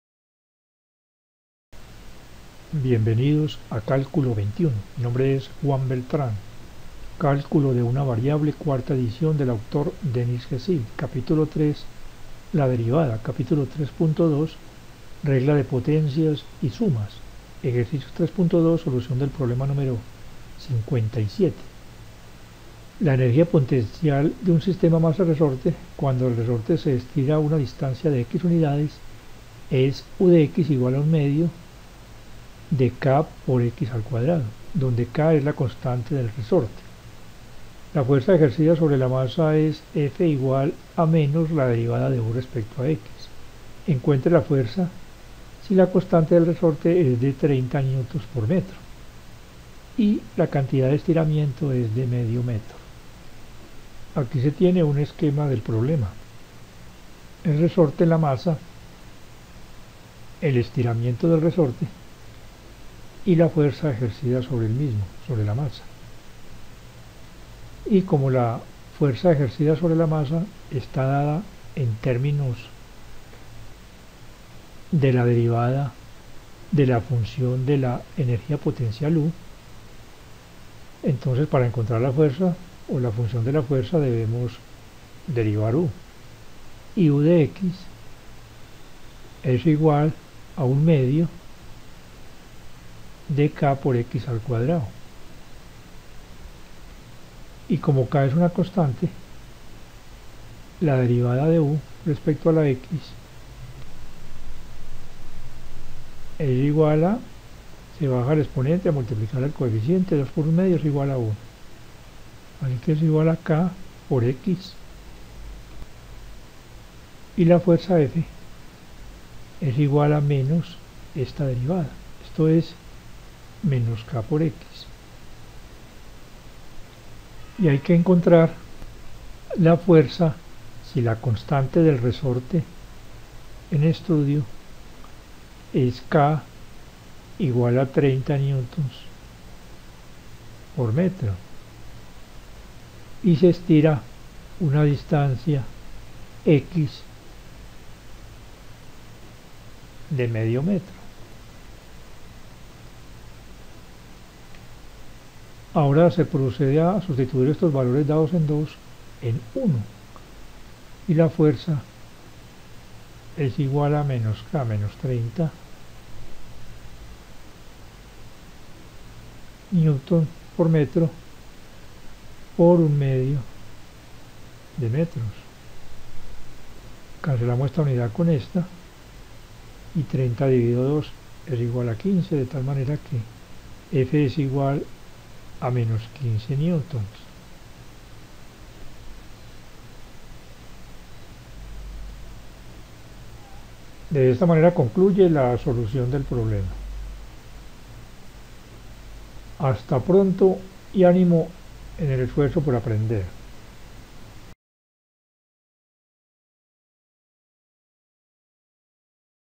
Problema de aplicación de la derivada a la Física. Cálculo I Zill 3.2_57
Se halla la fuerza ejercicda sobre un sistema masa-resorte dada la función de la energía potencial del sistema.
Solución de más problemas de los Ejercicios 3.2 del Cálculo I de Zill